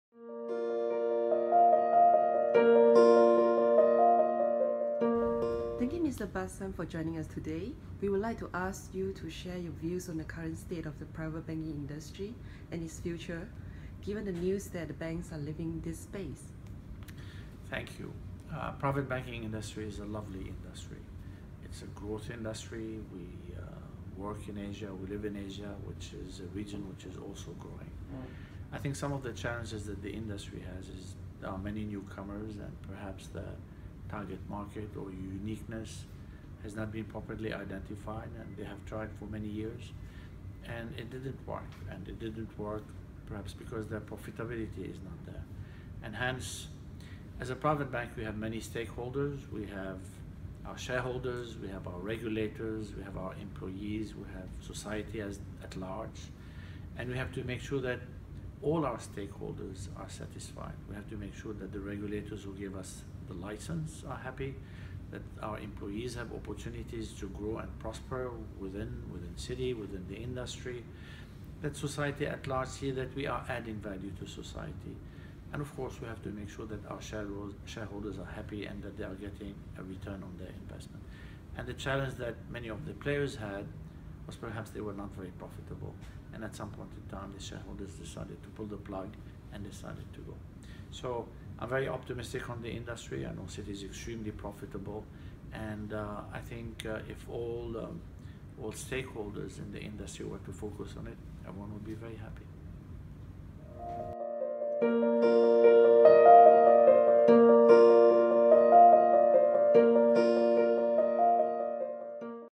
IBF CACS Connect @ Citi Private Banker
What does the future hold for the private banking industry given the news that more players are exiting this space? Bassam Salem, Managing Director, Region Head Asia, Citi Private Bank shares his views with IBF.